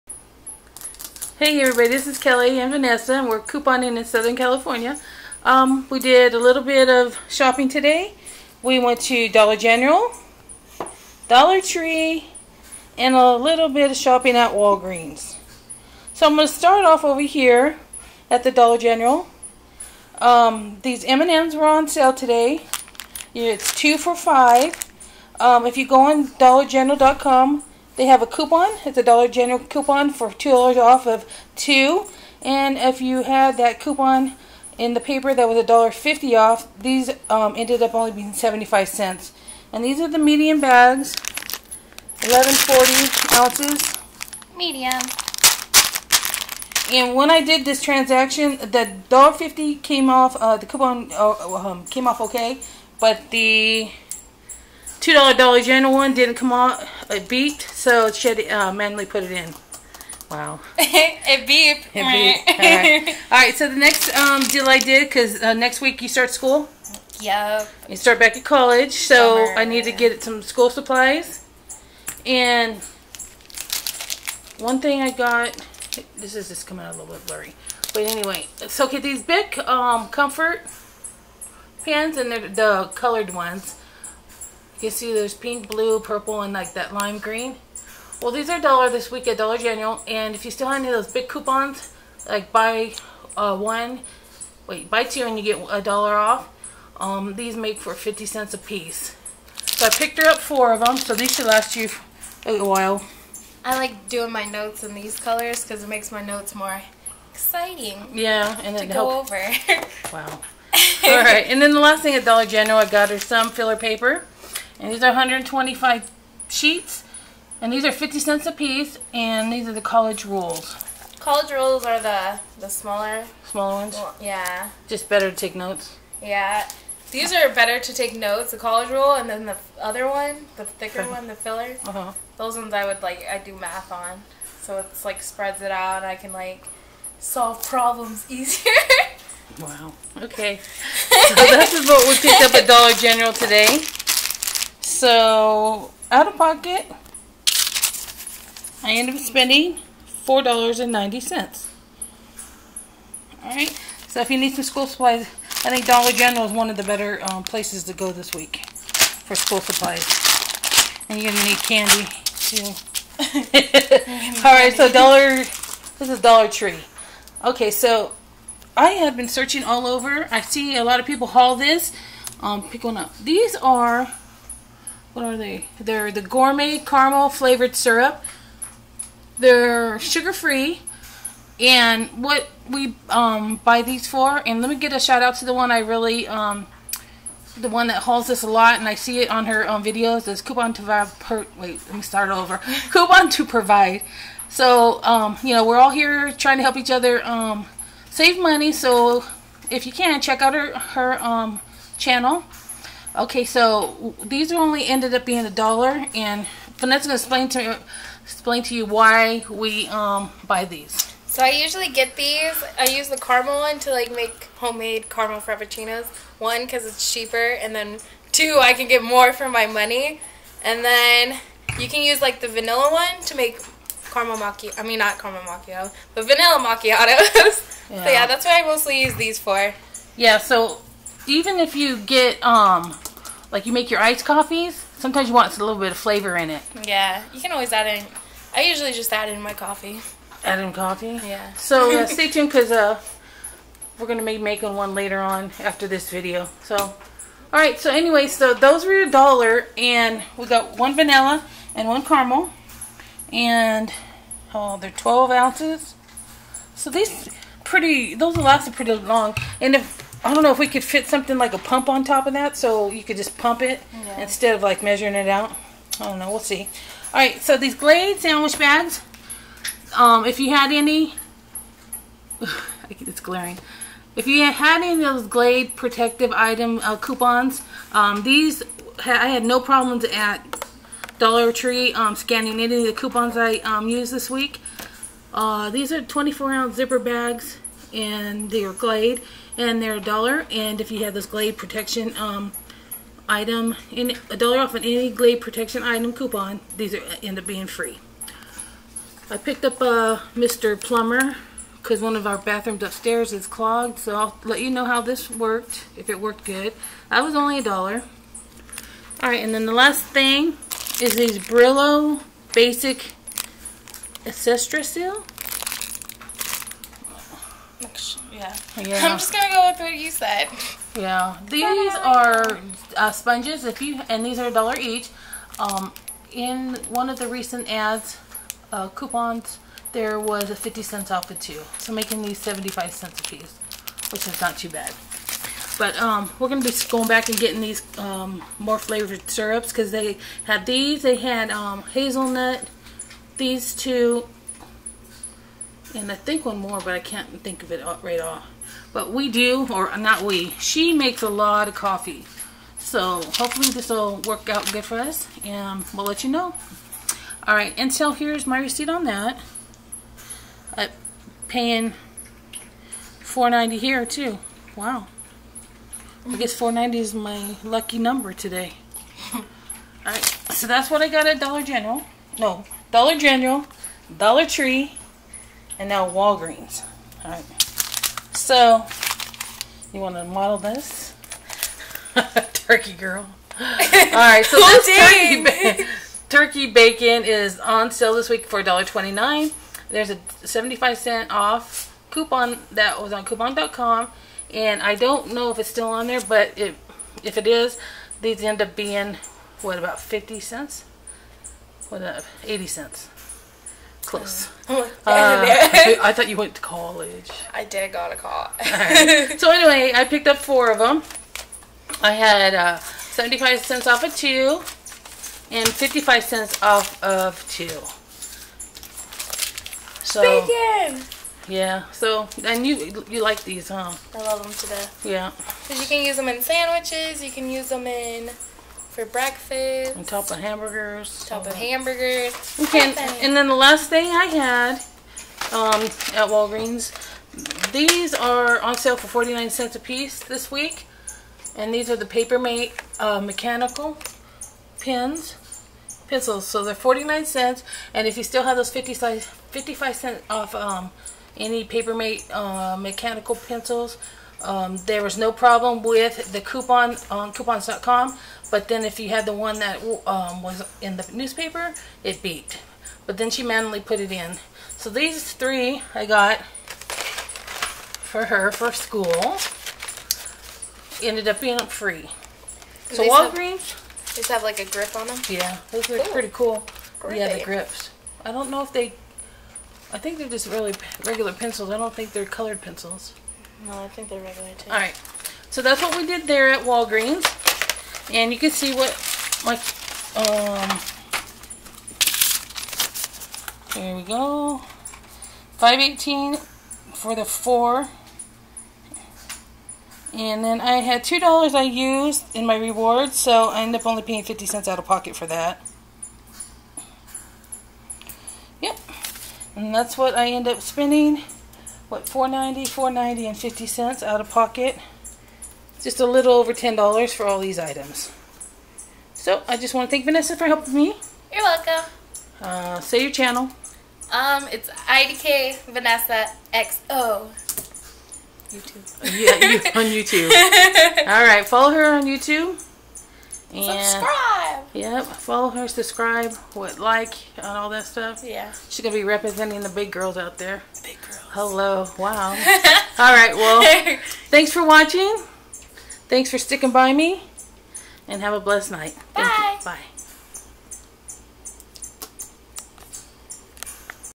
Couponing at Dollar Tree, Dollar General and Walgreens! 08/23/2015
Still shopping for school supplies for the kiddos and finally finding the flavored syrup at Dollar Tree!! !